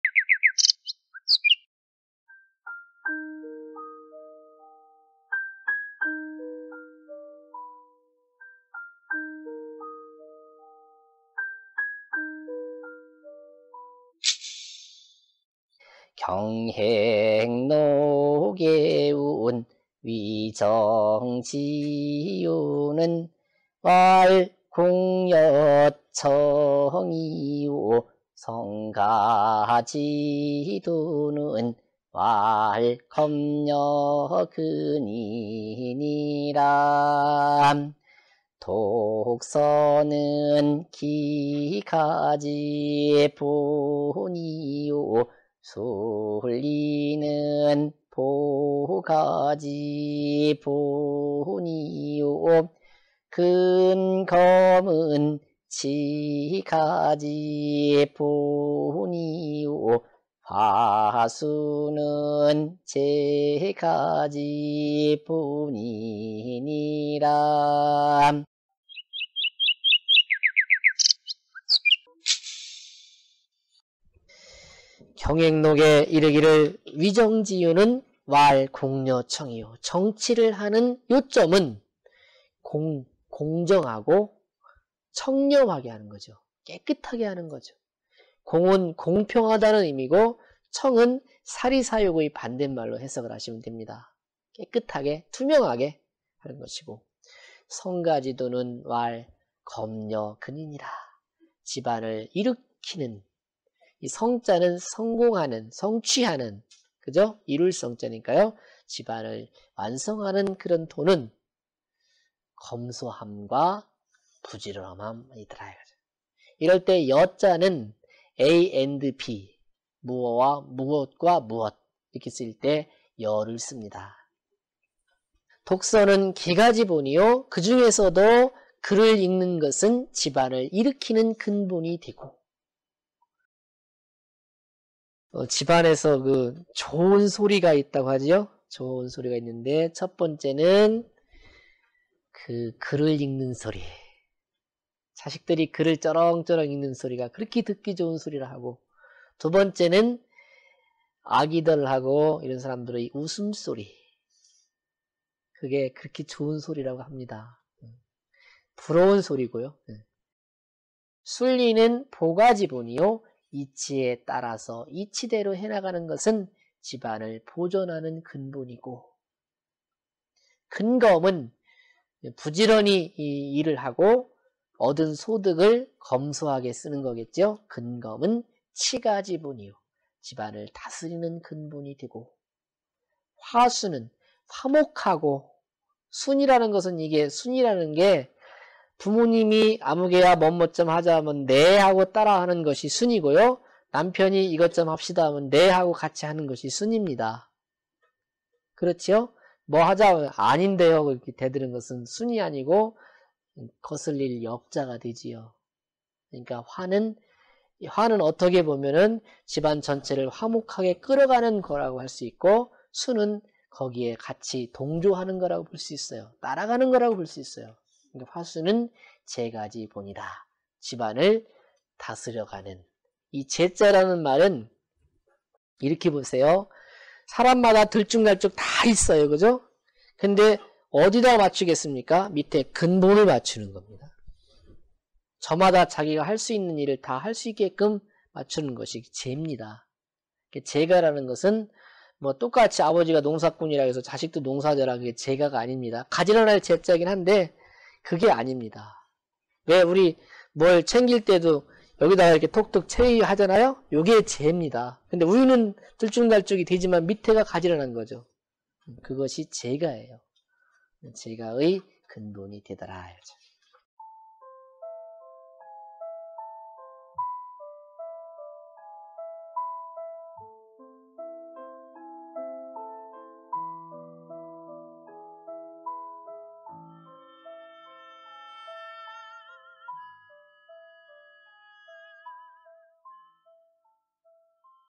명심보감 : 부지런함과 검소함은 집안을 성공으로 이끈다.
成家之道는 曰儉與勤이니라
집안을 성공으로 이끌려면,
검소와 부지런함으로 살아라.